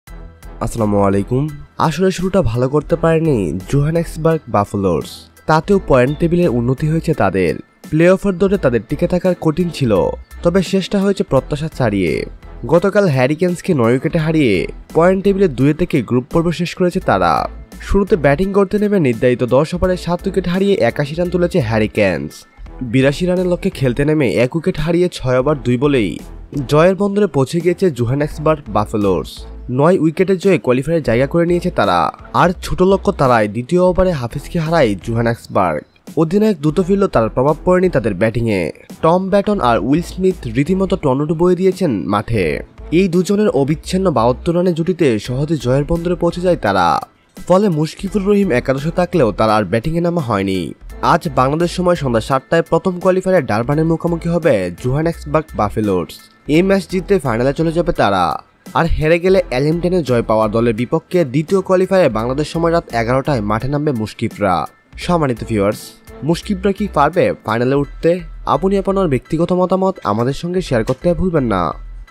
অবাক বিশ্ব! হাইভোল্টেজ ম্যাচে সহজেই বড় জয়ে কোয়ালিফাই নিশ্চিত করল মুশফিকরা T10 League
হাইভোল্টেজ ম্যাচে সহজেই বড় জয়ে কোয়ালিফাই নিশ্চিত করল মুশফিকরা